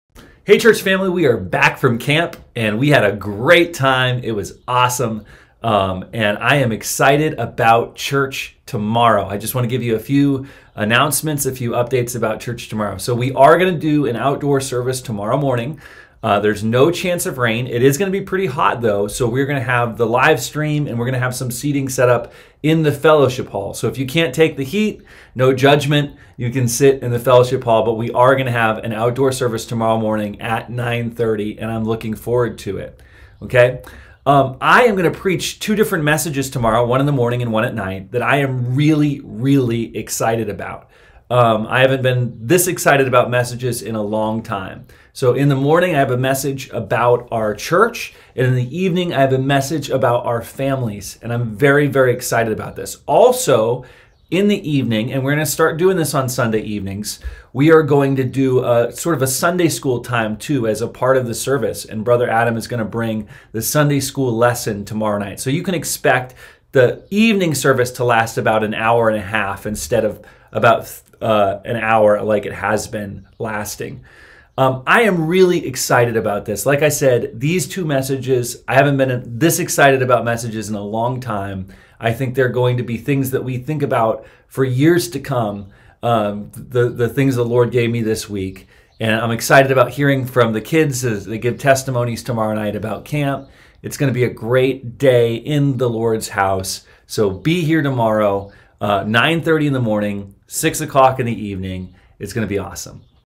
Church Announcements - July 26
Outdoor Service - 9:30AM (Livestream and AC seating in fellowship hall will be provided)
Evening Service - 6:00PM (We will include a Sunday School time, combined this week - expect the service to last until 7:30P